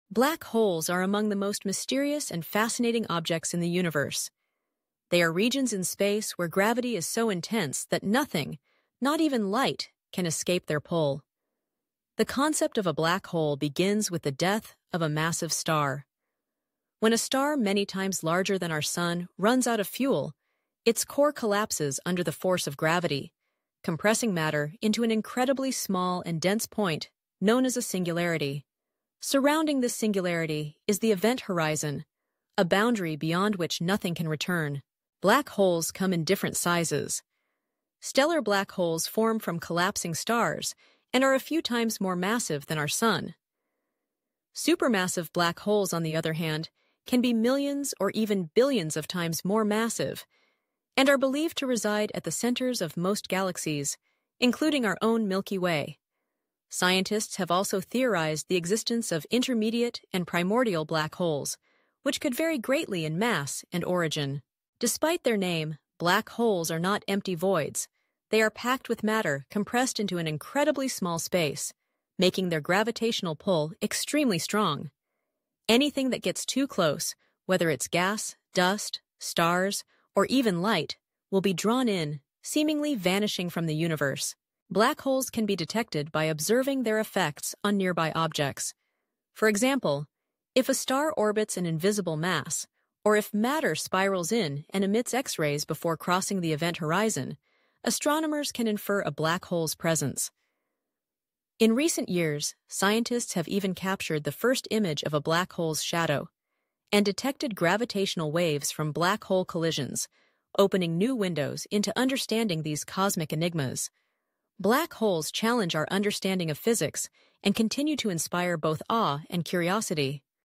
Black Holes Explained (Super Fast!)
Time stops. Light vanishes. Gravity goes wild. Discover what makes black holes the scariest objects in space – all in just a minute! ⏳🕳️